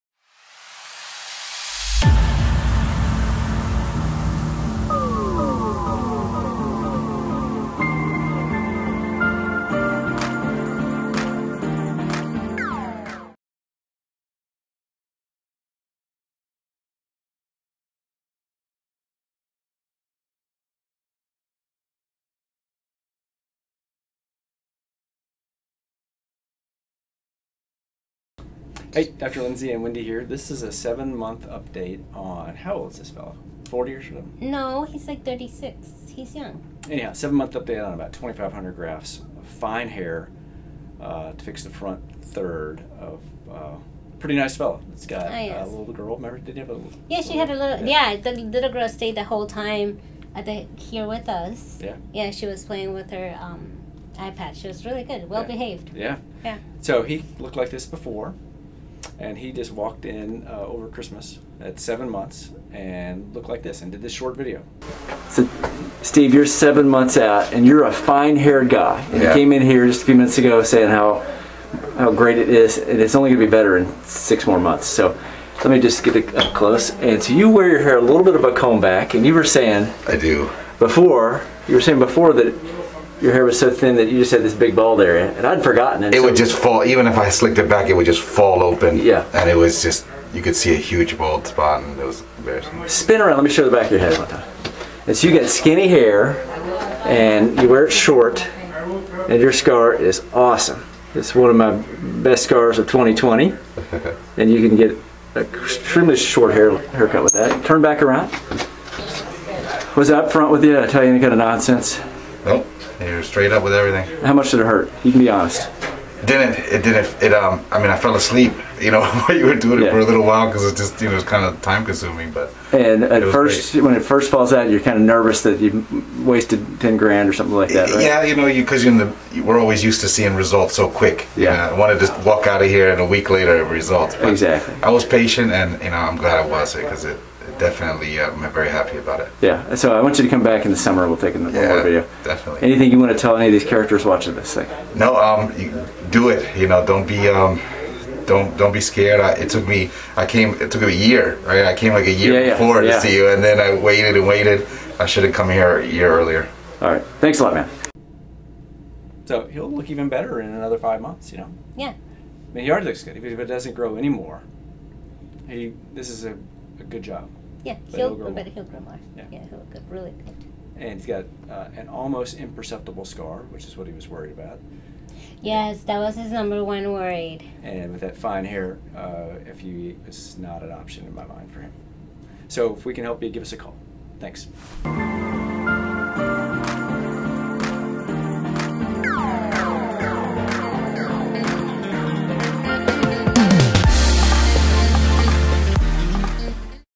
Early hair growth in fine hair 40 year old after hair transplant by Dr. Lindsey in McLean VA
Click The Link Below To Book Your Free Hair Restoration Consult Today!
Restore your hair with natural-looking results with Dr. Lindsey, world renowned hair doctor in Virginia.

1:00 preop hair transplant

1:30 discussion with hair restoration patient

2:00 fine hair and scar discussion